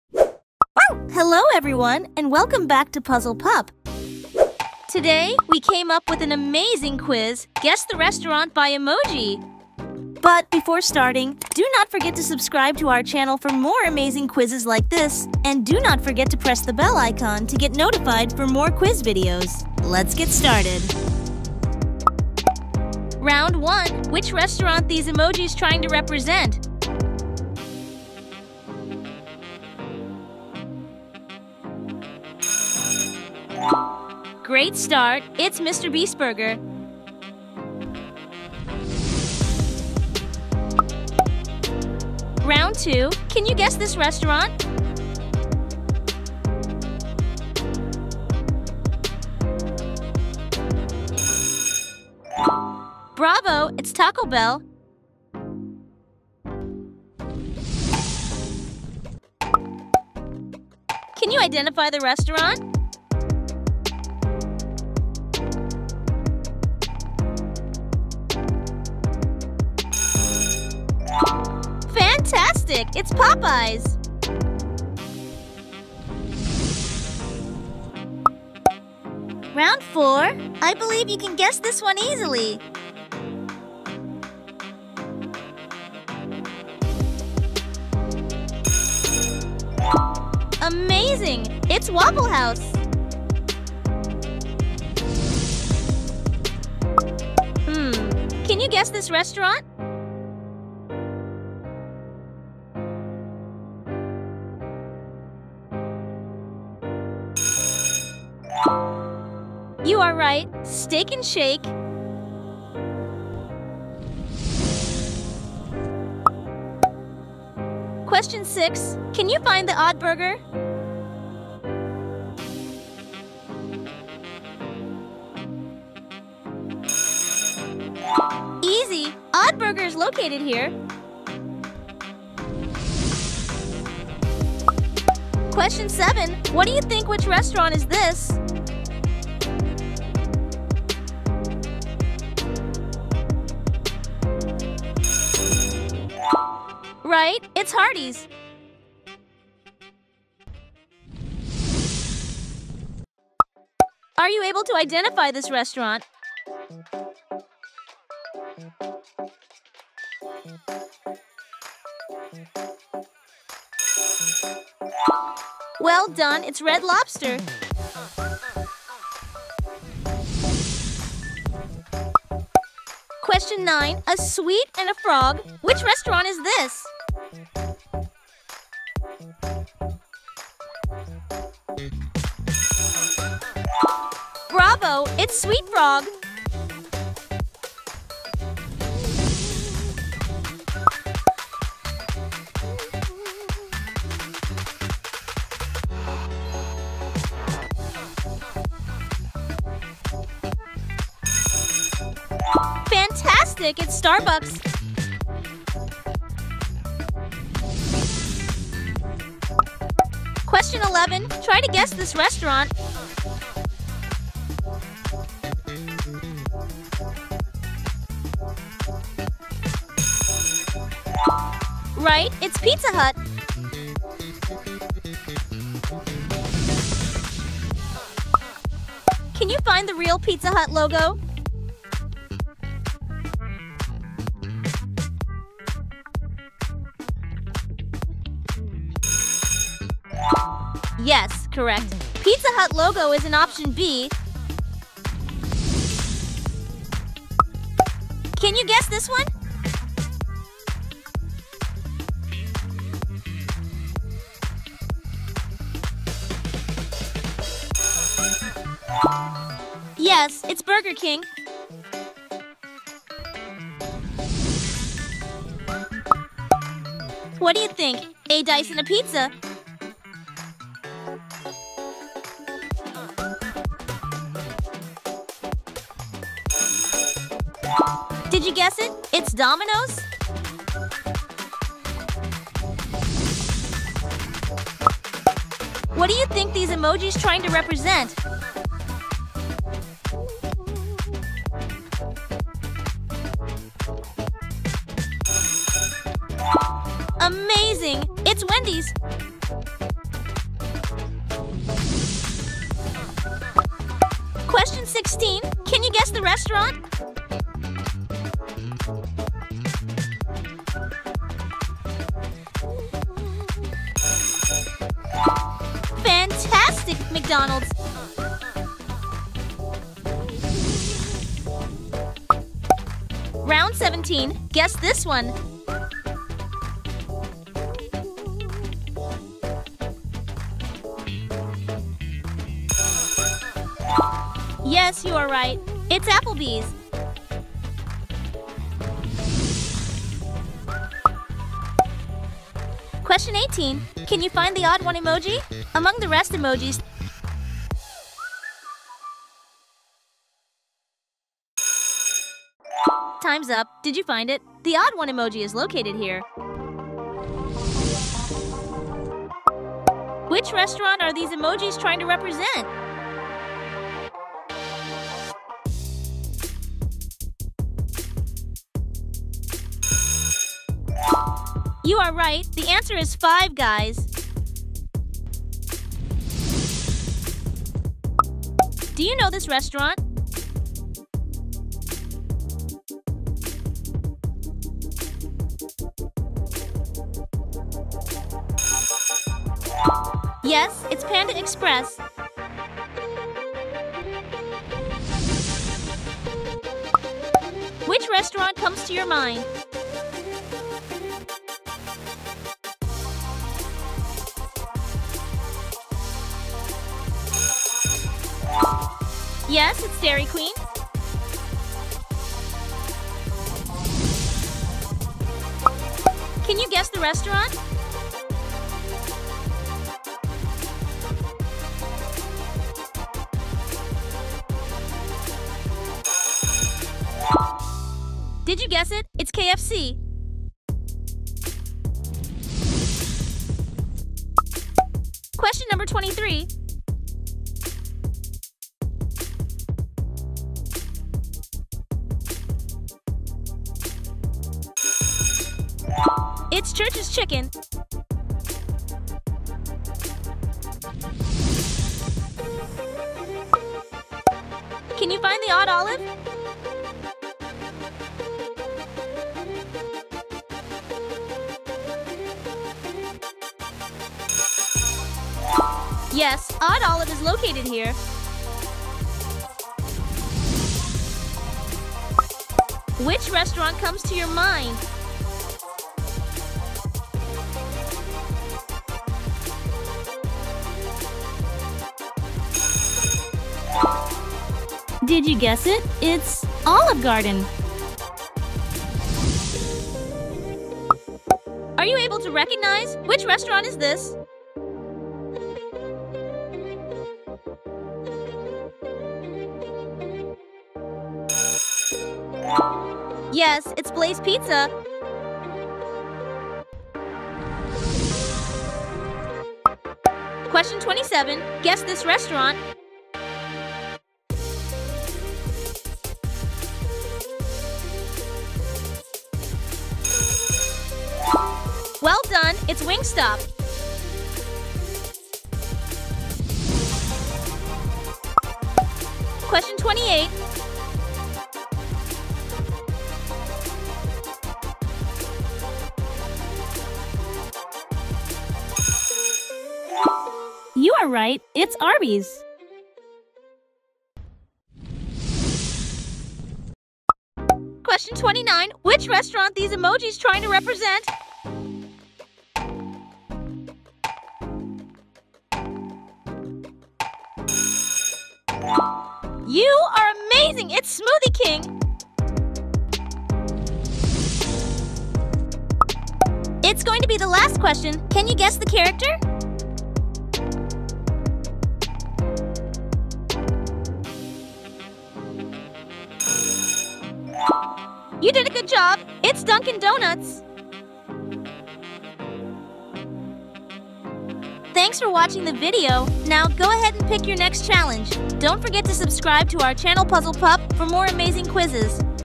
Guess the Fast Food Restaurant by Emoji? 🍔🍕| Fast Food Emoji Quiz | Puzzle Pup
Guess the Fast Food Restaurant by Emoji? 🍔🍕| Fast Food Emoji Quiz

In this video, we are playing a game called "Can You Guess The Fast Food Restaurant by Emoji? Fast Food Emoji Quiz".
In this game, you will have to guess the Fast Food Restaurant based on the suggested emoji we give you in 8 seconds. We have included 30 different Puzzles in this game, so the game is sure to be fun! 🍟🍔🌭